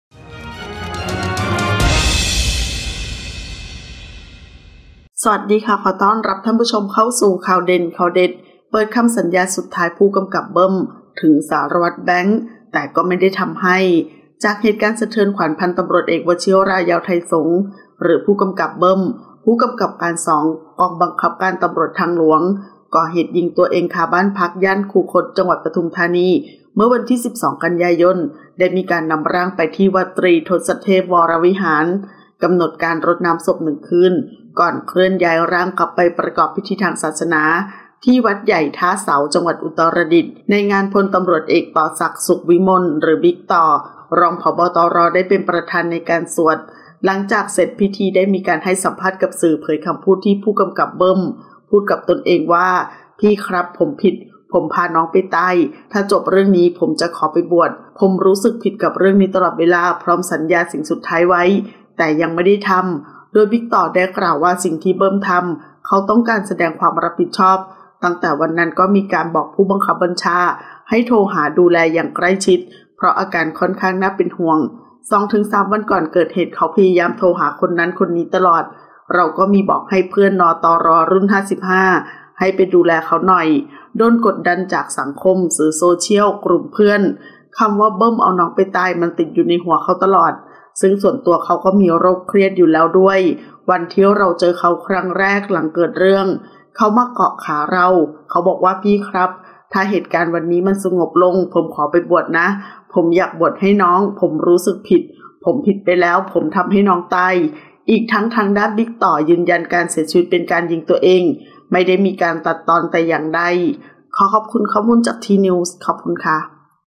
เปิดคำสัญญาสุดท้าย “ผกก.เบิ้ม” ถึง “สารวัตรแบงค์” แต่ก็ไม่ได้ทำให้
00:00 - Intro
00:10 - News
01:00 - Minute